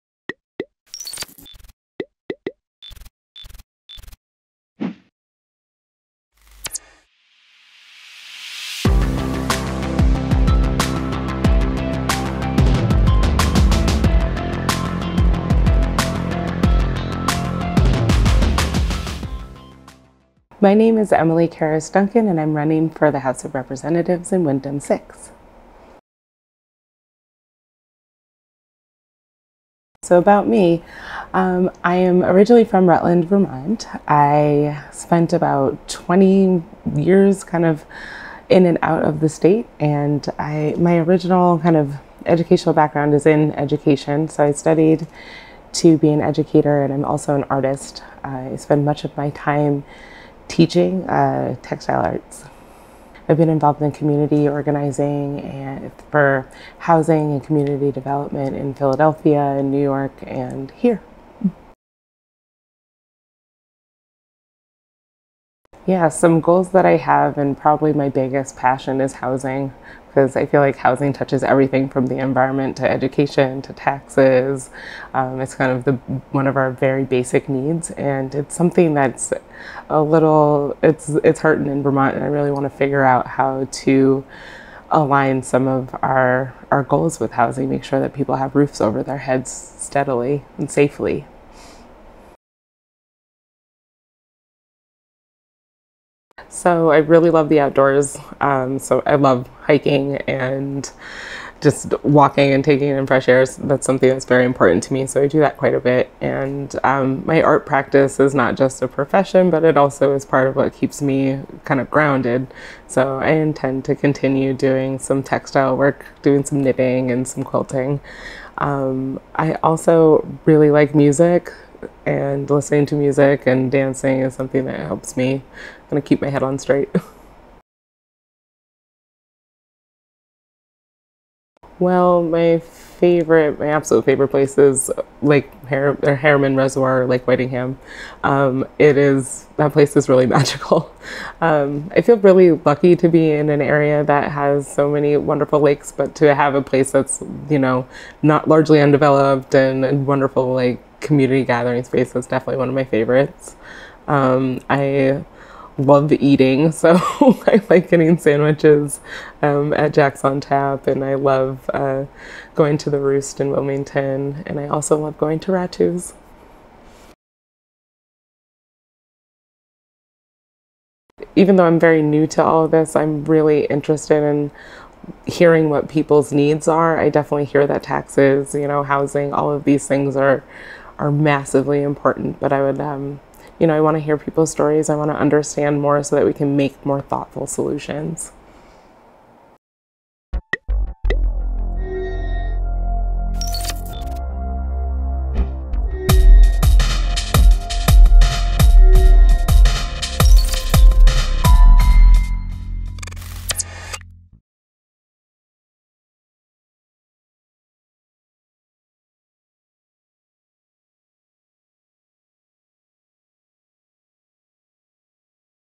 Beyond The Ballot: Emily Carris-Duncan, Windham-6 (D), 2024
In "Beyond The Ballot" BCTV brings in political candidates to learn more about them as people, instead of being reduced to political parties on a piece of paper. The candidates in this race are running in the 2024 general election for statewide and nationwide positions. Emily Carris-Duncan is running as a democrat for State Representative in Windham District 6.